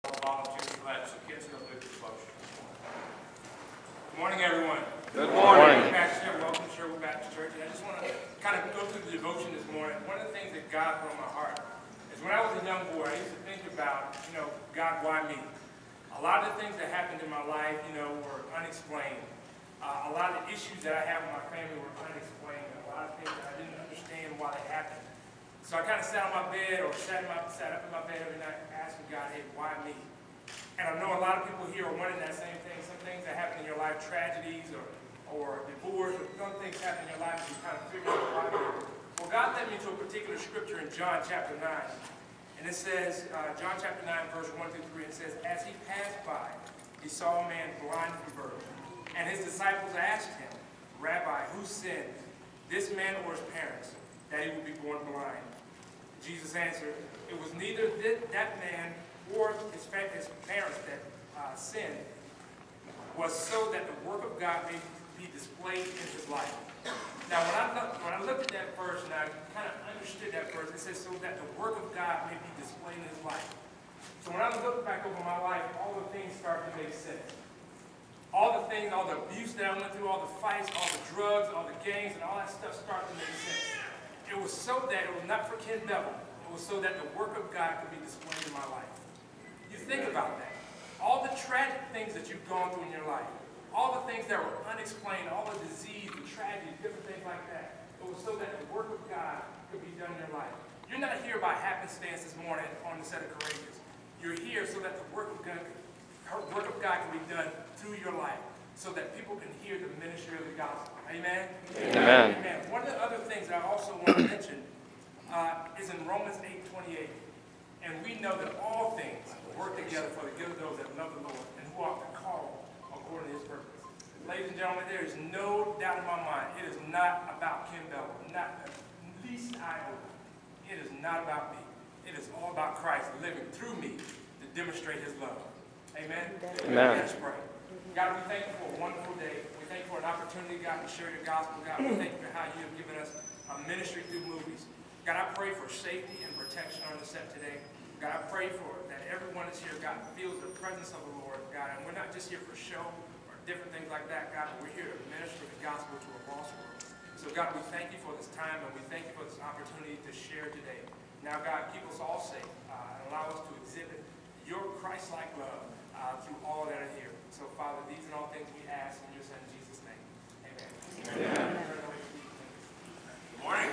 Actor Ken Bevel Leads the Morning Devotional (HD)
Prior to shooting the first scene of the day, actor Ken Bevel (who plays Nathan Hayes) leads the morning devotional before a crowd of media (including CMR). Ken is introduced by writer/producer, Stephen Kendrick.

Video Date: Tuesday, June 8, 2010 at Sherwood Baptist Church in Albany, GA